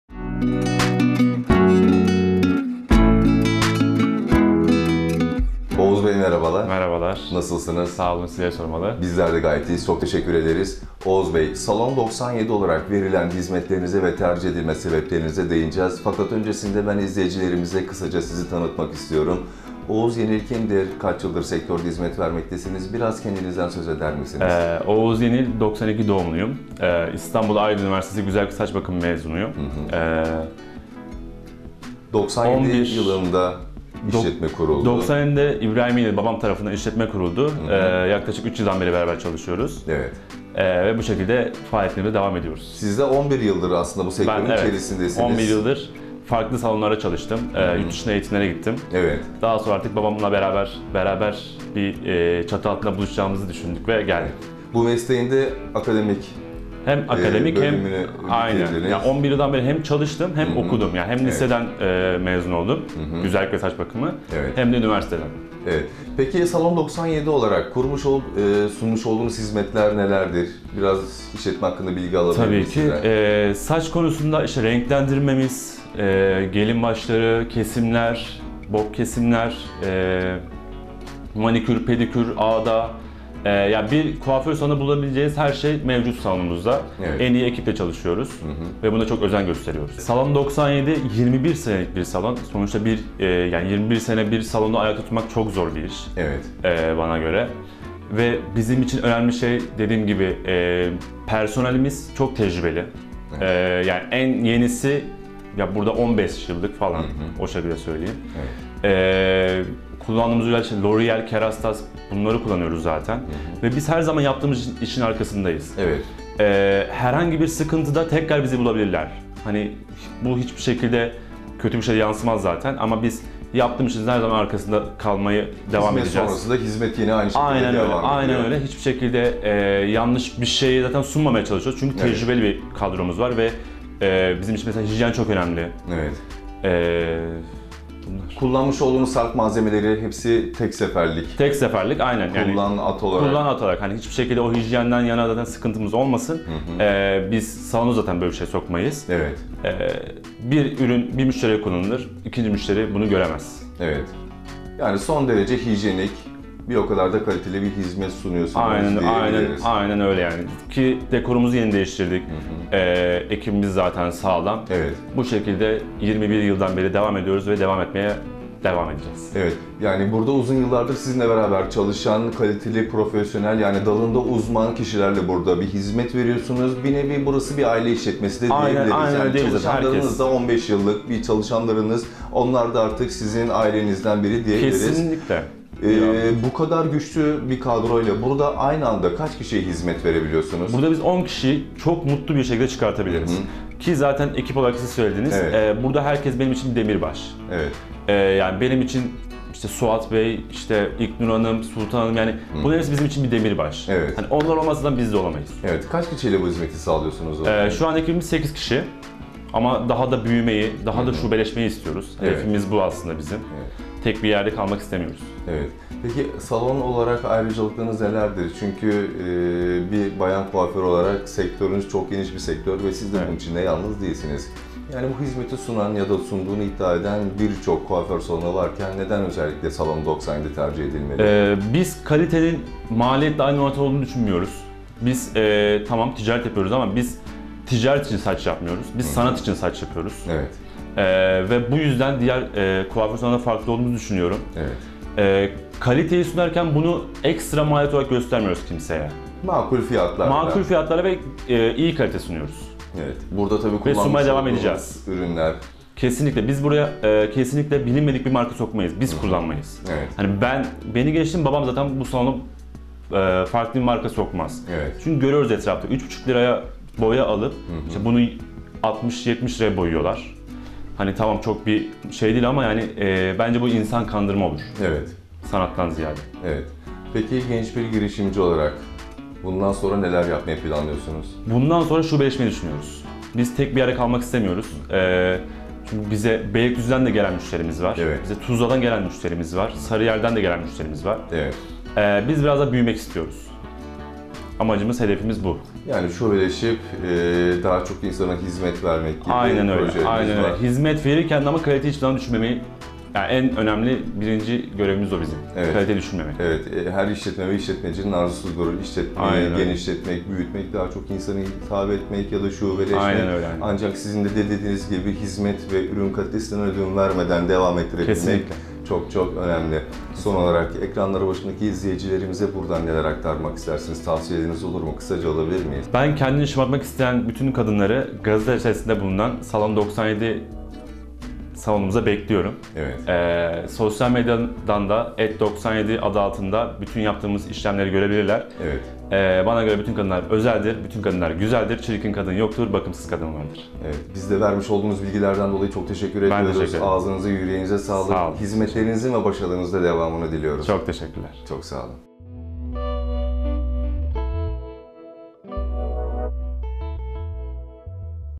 SALON DOKSANYEDİ - MASLAK/İSTANBUL
İstanbul’un nokta mekanlarından Maslak’ta hizmet veren Salon 97 sunmuş olduğu hizmetleri Kobi Özel’e anlattı.